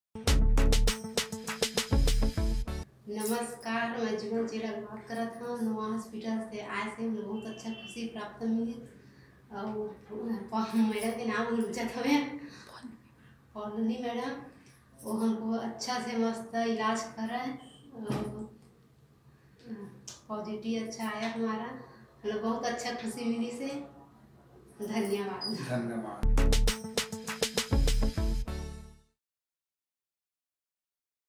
The couple conceived after IVF treatment by Dr. Paulami Dey, IVF specialist, Nova IVF Raipur
The couple took treatment from Dr. Paulami Dey, IVF specialist, Nova IVF Raipur and got a positive result of their treatment. They are very satisfied with the atmosphere of the clinic, the staff members, the doctor, and the services they received from Nova IVF.